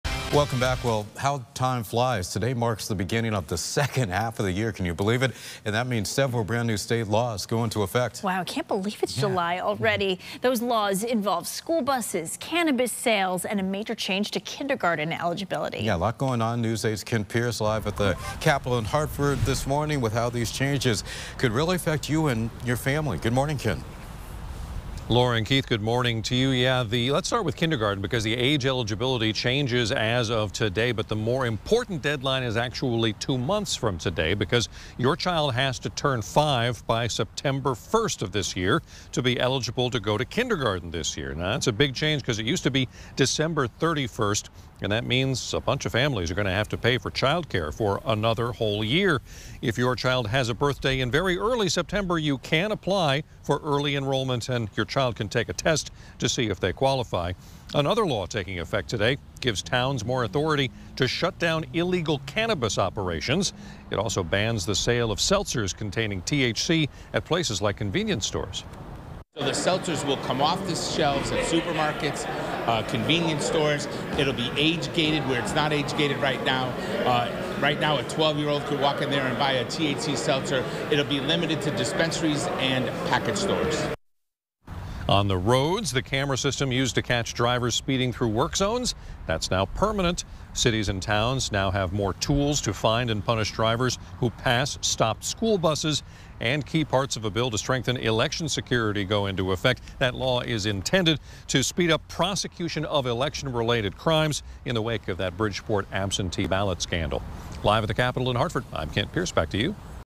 New Connecticut laws protecting children and targeting ballot fraud take effect July 1
When the 2024 legislative session ended on May 8, state lawmakers sent 175 bills to the Governor’s desk. Lamont vetoed two and signed the other 173 into law.